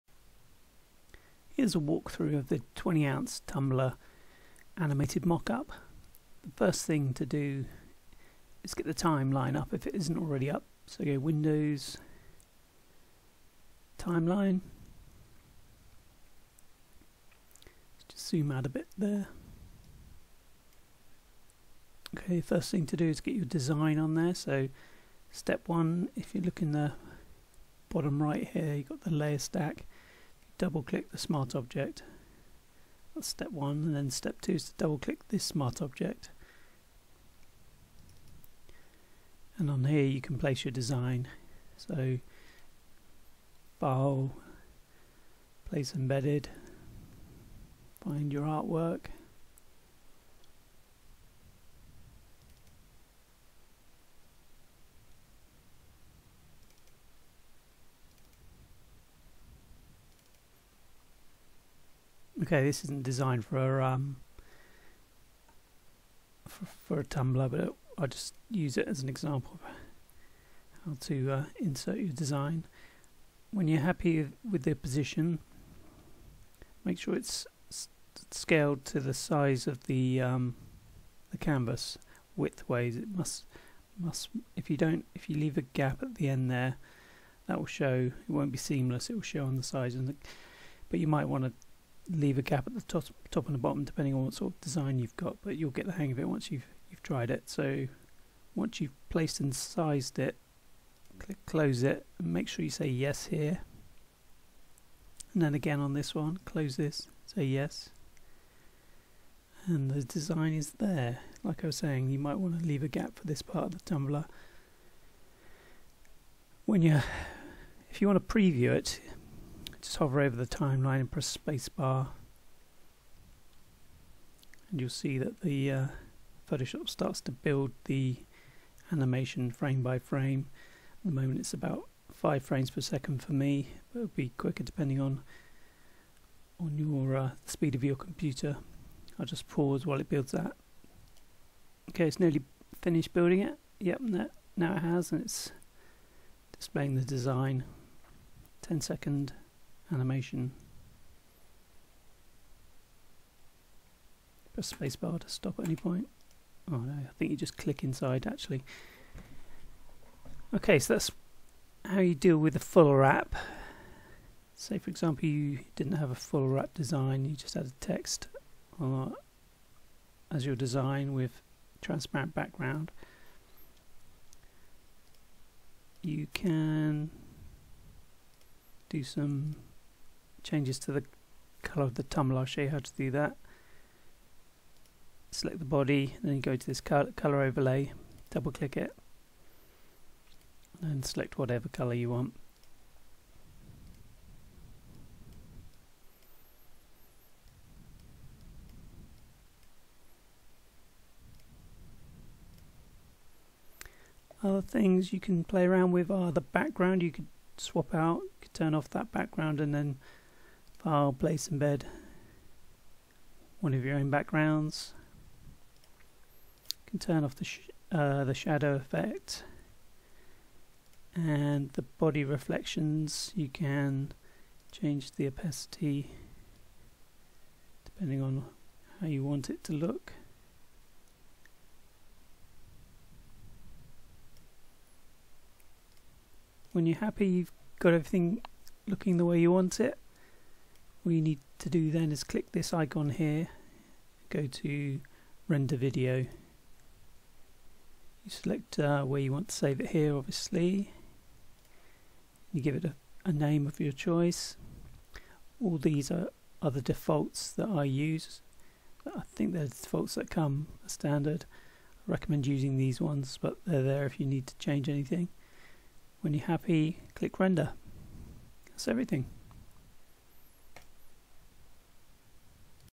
20oz tumbler mockup animation
Photoshop animated 20oz tumbler mockup. 360 degrees lasting 10 or 15 seconds.